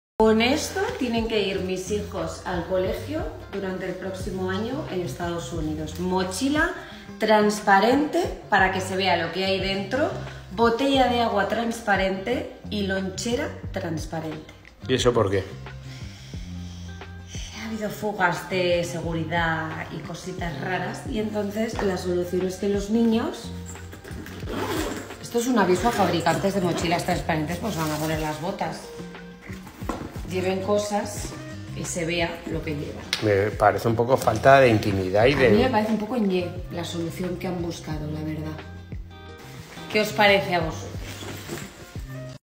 Una ESPAÑOLA muestra el tipo de MOCHILA que se lleva en EEUU al COLEGIO | Diario AS
Nury Calvo ha revelado que sus hijos deberán llevar una mochila transparente el próximo curso para que se vea todo lo que hay dentro